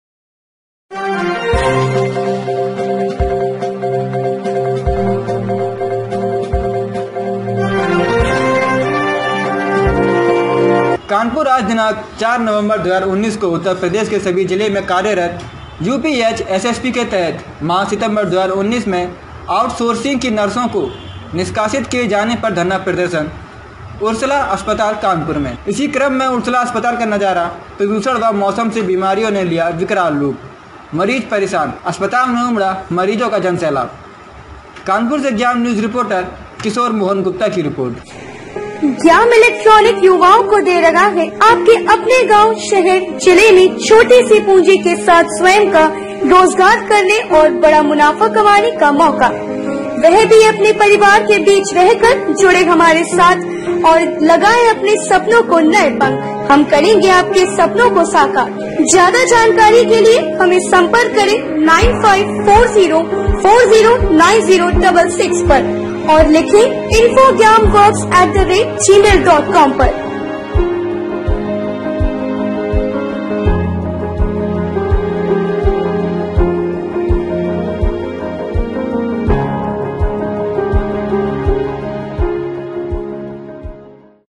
GyamNews _कानपुर-आज दिनांक 04-11-19 को उत्तर प्रदेश के सभी जिले में कार्यरत यूपीएचएसएसपी के तहत.👇
*_कानपुर-आज दिनांक 04-11-19 को उत्तर प्रदेश के सभी जिले में कार्यरत यूपीएचएसएसपी के तहत माह सितंबर 2019 में आउट सोर्सिग की नर्सो को निष्कासित किए जाने पर धरना प्रदर्शन उसर्ला अस्पताल कानपुर में_*

*_इसी क्रम में उर्सला अस्पताल का नजारा प्रदूषण व मौसम से बीमारियों ने लिया विकराल रूप मरीज परेशान अस्पताल में उमड़ा मरीजों का जनसैलाब_

कानपुर से ज्ञाम न्यूज रिपोर्टर किशोर मोहन गुप्ता कि रिपोर्ट